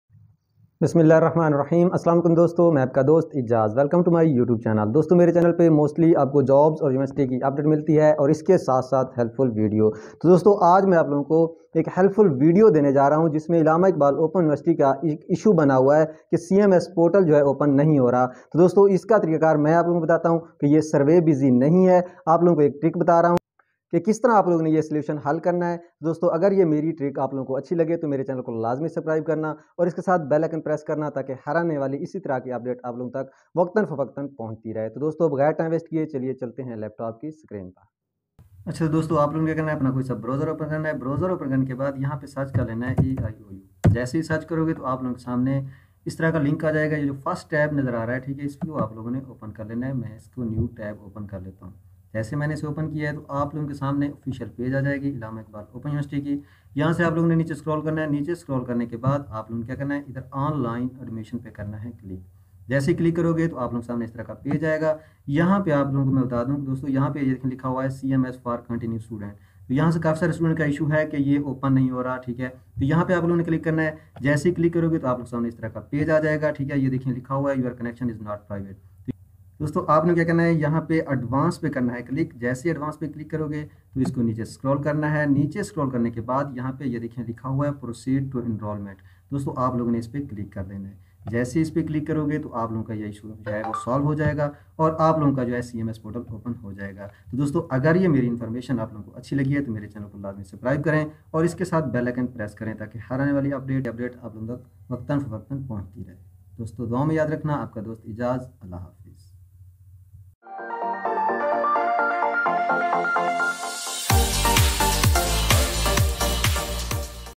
aiou CMS Portal link issue solved | Portal issue solution | cms msla hal ho gia I cms error solved
aiou k cms ka portal open ni ho rha tha to ab us ka hal mil gia h bss video watch krain
is video me is ko btao ga kis trah ap log apna issue solve lr skty hn mukmal malomaat h
how to solve cms portal issue | cms portal error issue solve

Allama iqbal open univrsity ka portal open ho rha h ab student ki pryshan ka hal nikal aya h

Anjum tips and trick